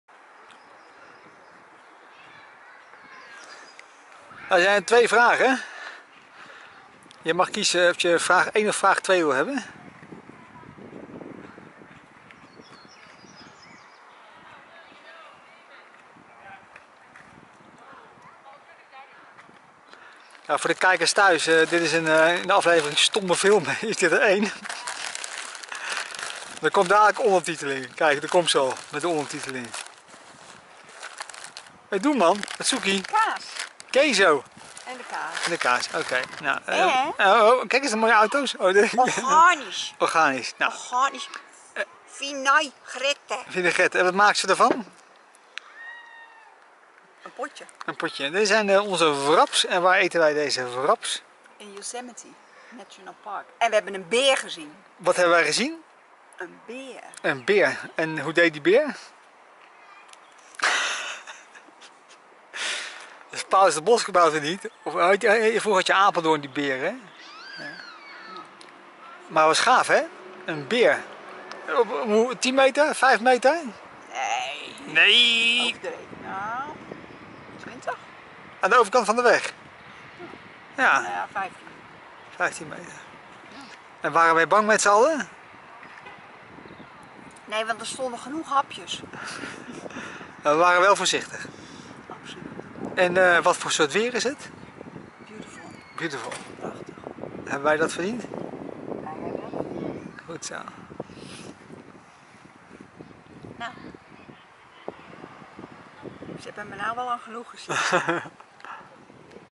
145. Marleen praat na over de zwarte beer
Marleen showt lekker eten en praat na over de zwarte beer